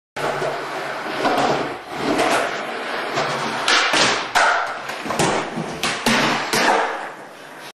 Pats house
Patrick and kurtis.
I was just testing out the focus ring on the trv900